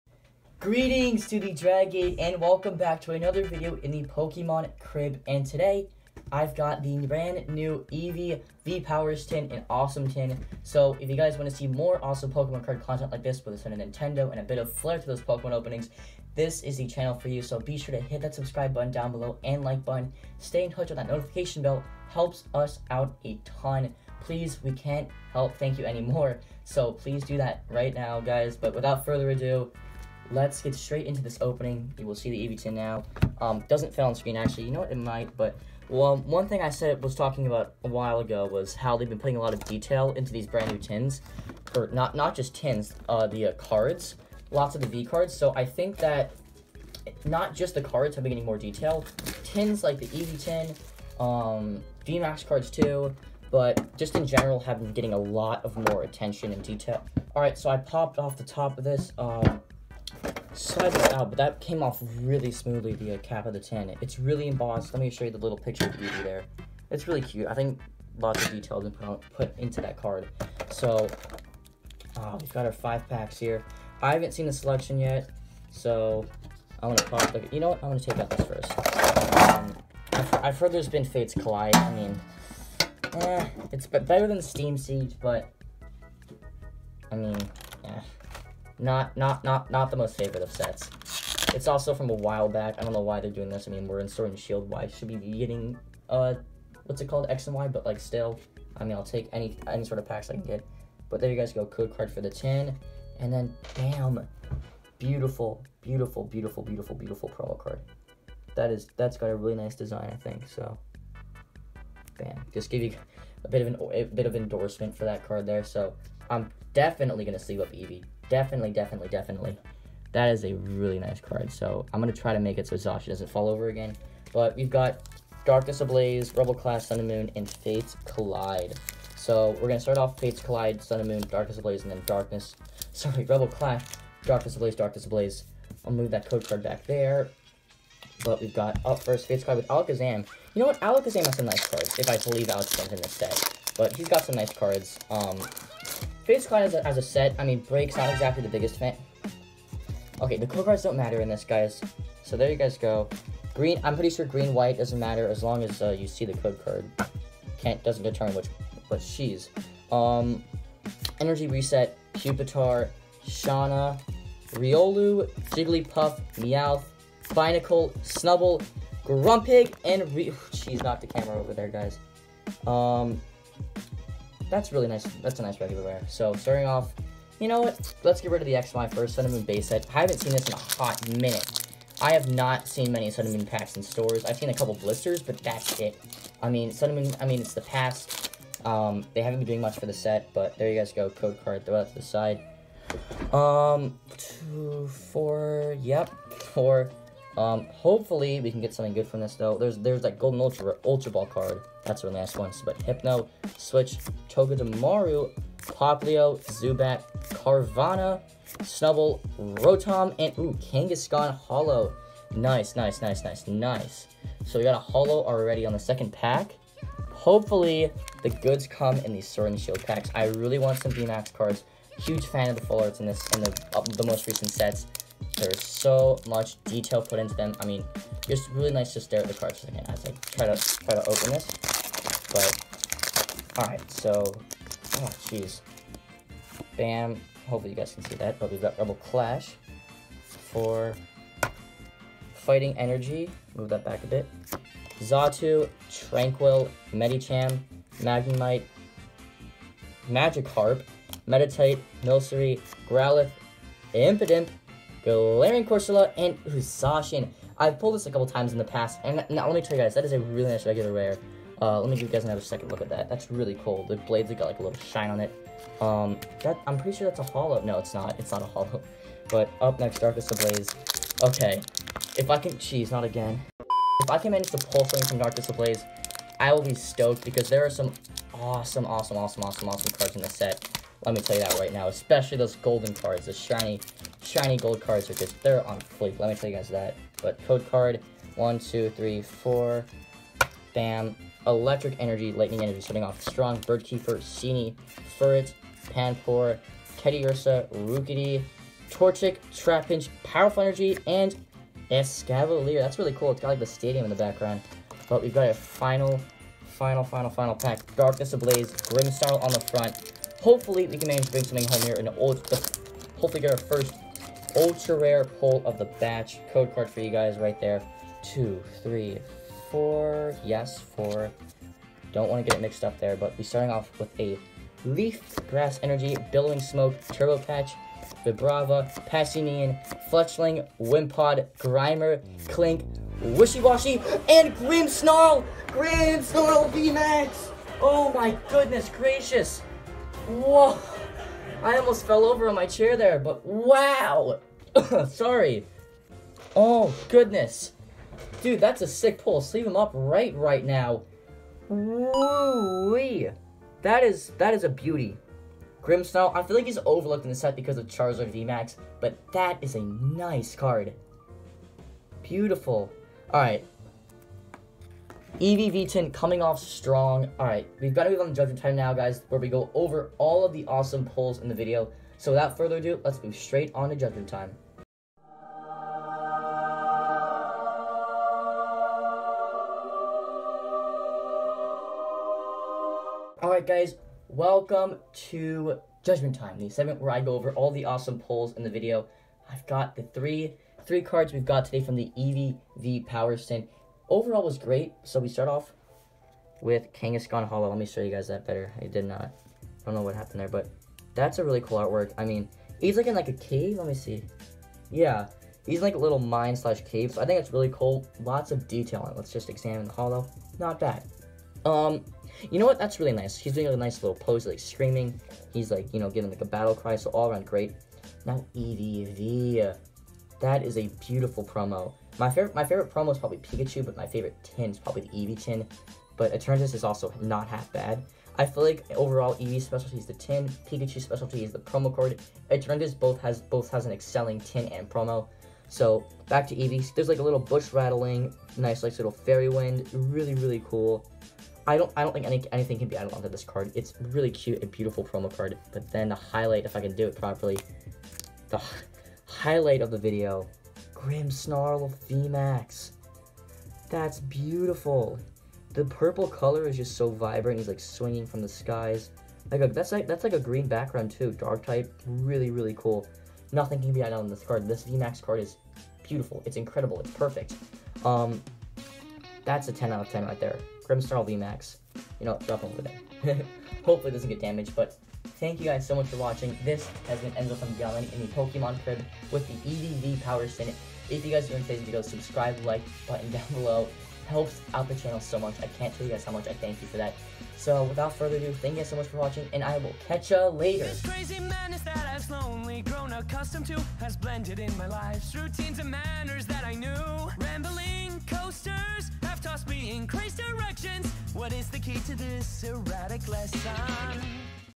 *NEW* Pokémon Eevee V Powers Tin Opening
The NEW Eevee V Powers Tin released during the Darkness Ablaze era has one of the most detailed tin designs, and promo card designs of all time. The question is, how good are the pull rates? Watch till the end to find out!

Welcome to Pokémon Crib, A.K.A My Room. Here you will find all sorts of awesome Pokémon related content!
Until then, I'll catch ya later!